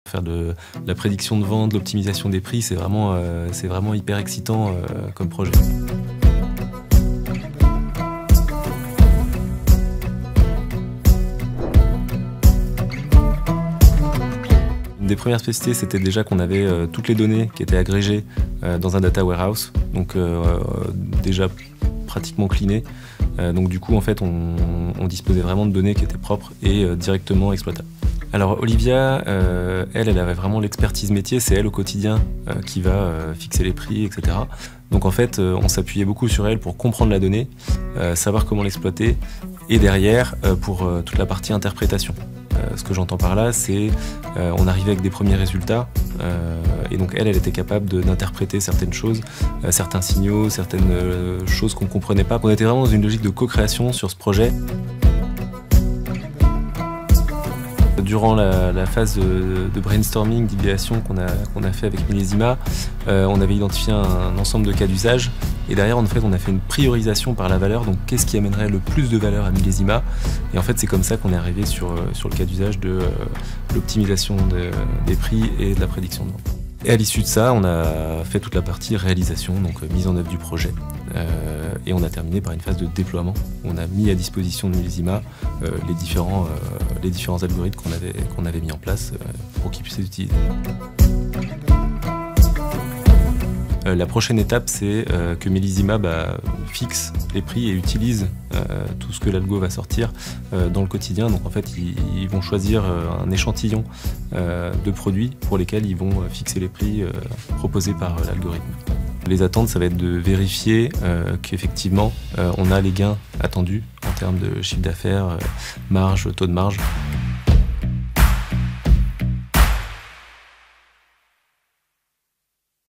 Talan x Millesima : Cyril Sacenda, manager Data/IA Talan (english subtitles)
Quand l'IA rencontre le monde du vin / When AI meets the wine industry.